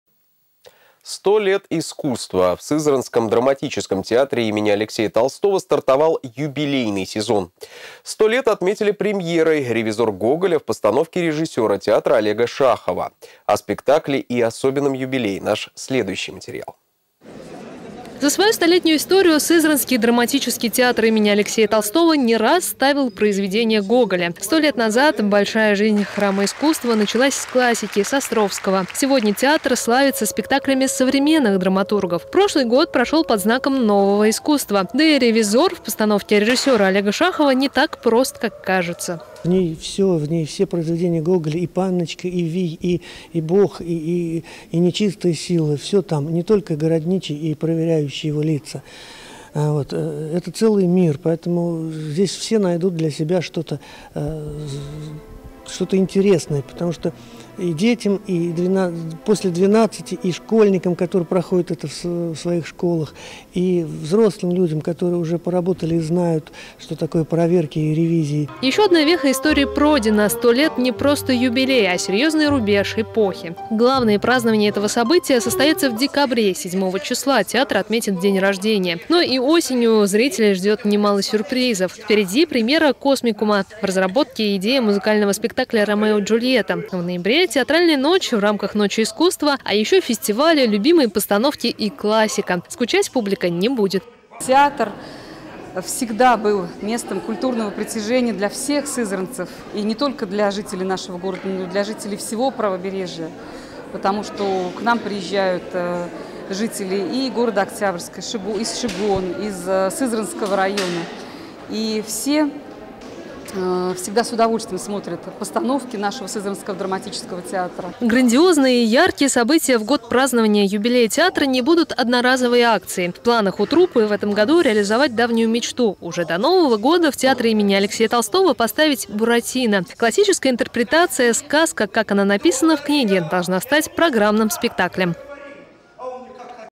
Сызранский драмтеатр начал юбилейный сезон
100 лет искусства. В Сызранском драматическом театре имени Алексея Толстого стартовал юбилейный сезон. Вековой юбилей отметили премьерой - "Ревизор" Гоголя в постановке режиссера театра Олега Шахова. О спектакле и особенной дате - наш следующий материал.